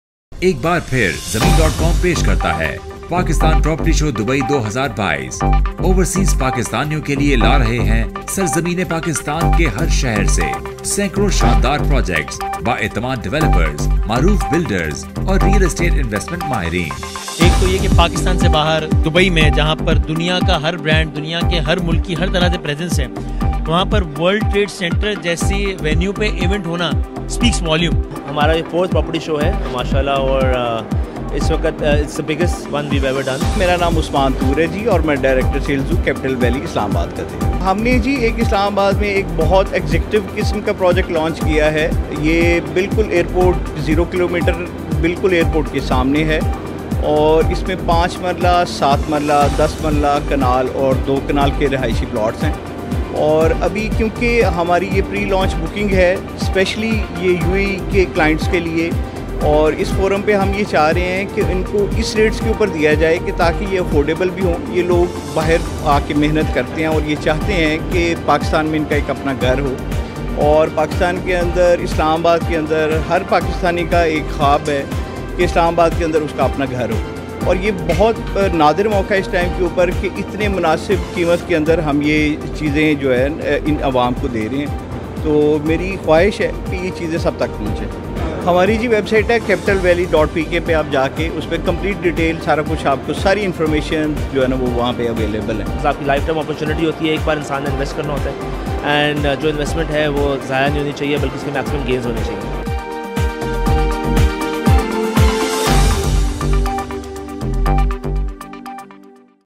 Capital Valley near Islamabad Air Port Plot Sale Purchase : Zameen.Com Pakistan Property Show Dubai
Trade Fair : Pakistan Property Show Dubai by Zameen.Com
Company : Capital Valley Islamabad
Country : Pakistan

ABOUT CAPITAL VALLEY ISLAMABAD

A Real Estate Project packed with all kinds of facilities means nothing if it is not readily accessible from the main points of the city. Considering this important aspect of the location, Capital Valley Islamabad is an unparalleled housing society in twin cities.

It is zero kilometres away from New International Airport Islamabad and just a few minutes drive away from the main Kashmir Highway and Zero Point Islamabad.

Capital Valley Islamabad is located at a highly accessible point where the major landmarks are only a few kilometres away.

Islamabad International Airport – 0 km
Thalian Interchange – 5 Minutes
Motorway M2 – 5 Minutes
G-13 – 20 Minutes
Srinagar Highway – 10 Minutes
Peshawar Road – 15 Minutes
Quaid e Azam International Hospital – 22 Minutes
Saddar – 30 Minutes
Jinnah Super Market – 34 Minutes
Ring Road – 3 Minutes

CAPITAL VALLEY ISLAMABAD PROJECT IS EXECUTED BY

Capital Valley Islamabad is executed by Capital Valley which boasts of consummating several construction and development projects in the past. The Urban Solutions Pvt Ltd is the town planner of Capital Valley Islamabad. They have earned a reputable name in the country’s real estate market. They have been entrusted with the development consultancy by big names like DHA, CBR, The Grand City Kharian, and Fazaia Housing Scheme. Green Thumb is also taking an active part in the landscape mapping of the society and the housing society will be exceptional in ensuring more green spaces than any other housing society in the region.